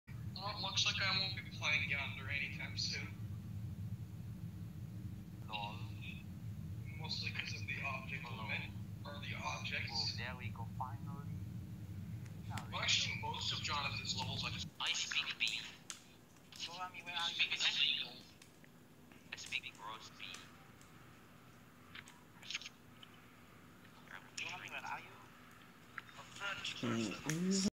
I knew it! JonathanGD uses hacks!
He exceeded the object limit and also verified a level with a start position. But his levels are amazing. And this is how he made Dark Travel and others so beautiful. BTW, I forgot to prove that he bypassed scale limit of the D block in the wave part halfway in Biru